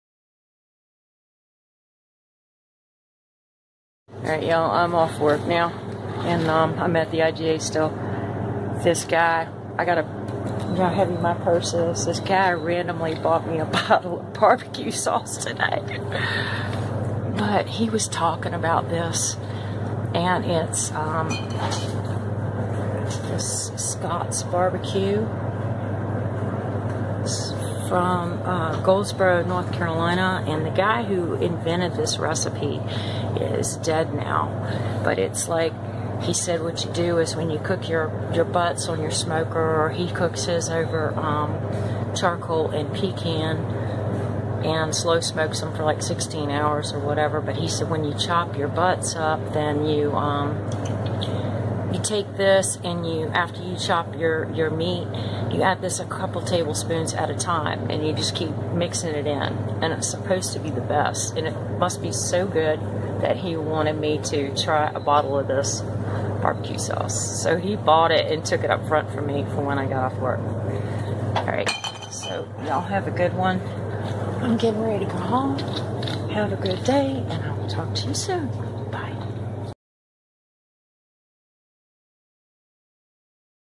IT Must Be that GOOD
A customer recommendation for Scott's Barbecue Sauce, made By Scott's Sauce Co in Goldsboro ,NC
phone number at Scott's is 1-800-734-SAUCE